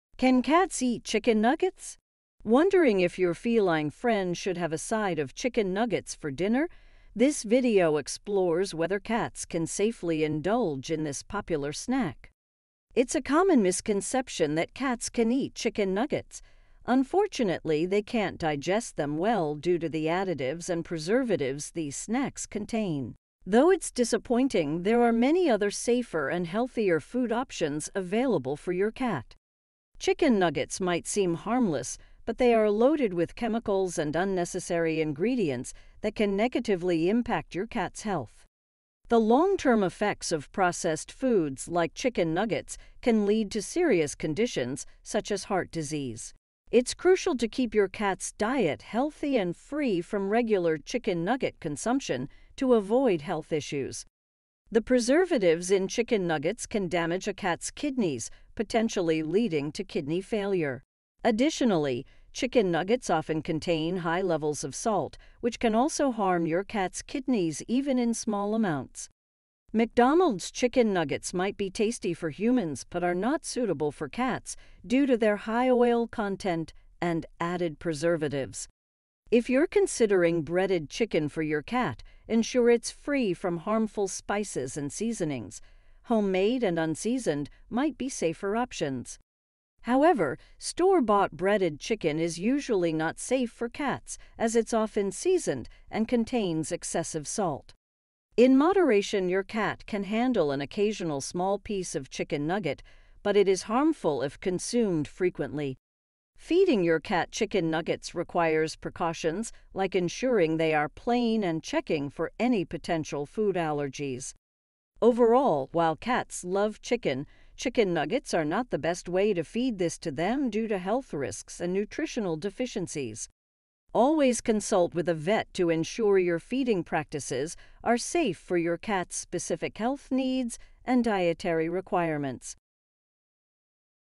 Can Cats Eat Chicken Nuggets | Top Cat Breeds
Curious if your feline friend can indulge in some chicken nuggets? 🐱🍗 Join us as we explore whether it's safe for cats to munch on this popular human snack!

From nutritional considerations to potential risks, we've got you covered. Watch till the end for expert insights and tips to keep your kitty happy and healthy!